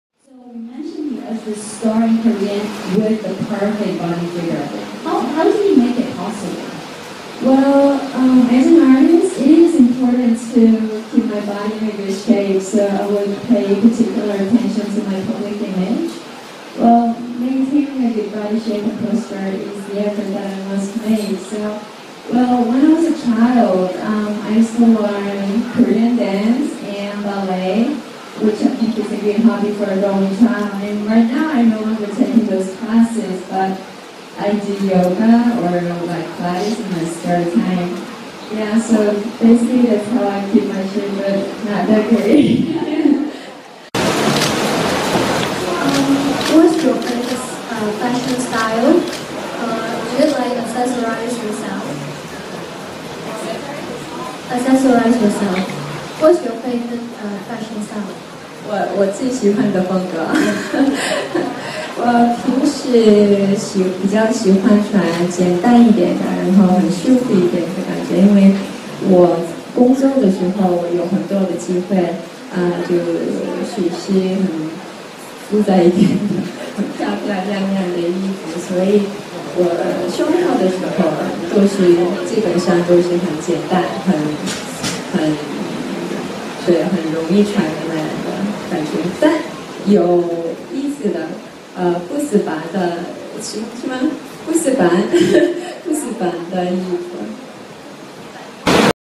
이다해 영어실력(리바이스 행사장)
이다해가 리바이스 행사장에서 출중한 영어실력을 뽐내고 있다.